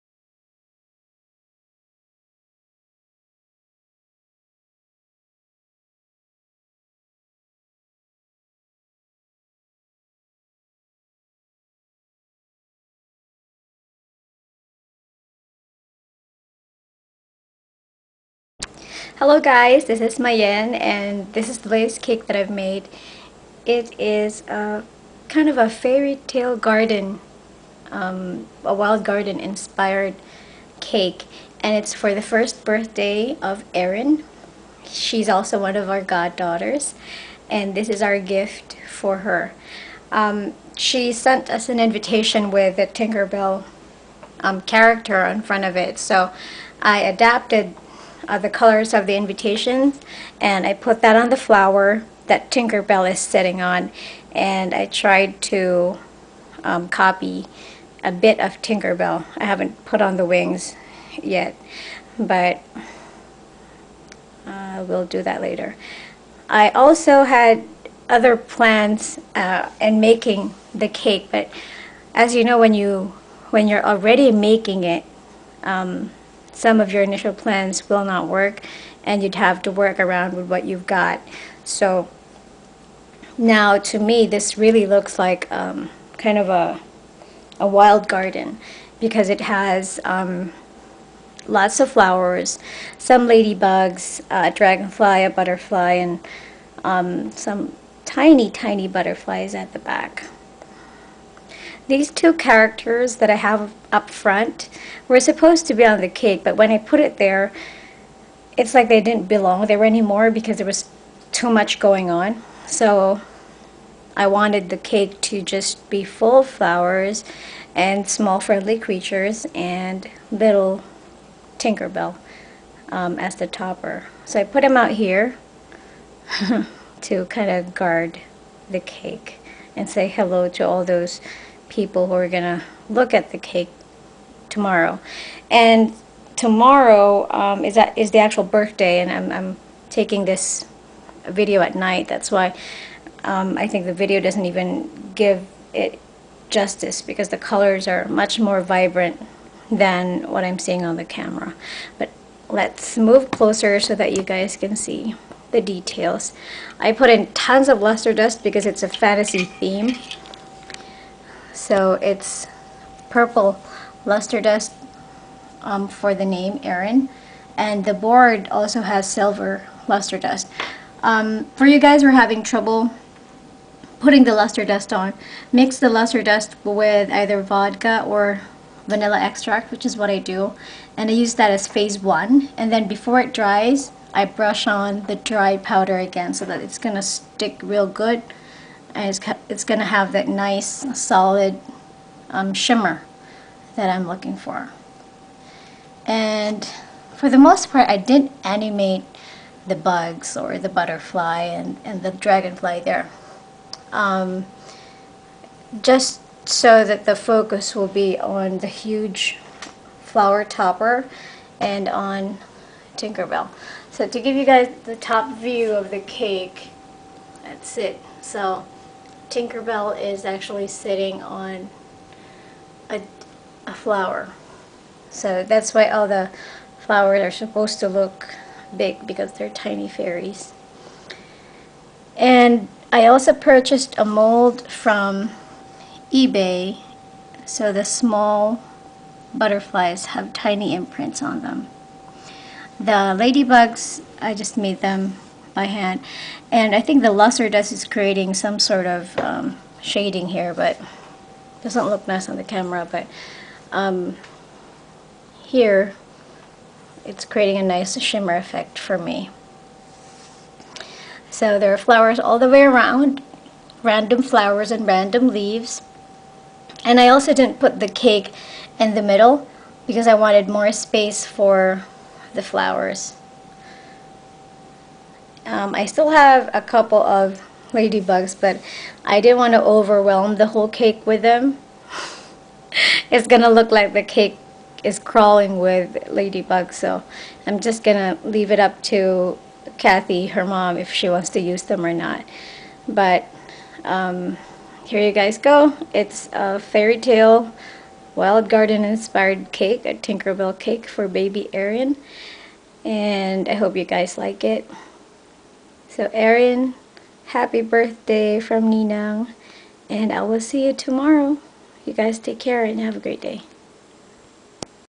Fairy Themed Fondant Birthday Cake
2 tier cake , all figures were made from gumpaste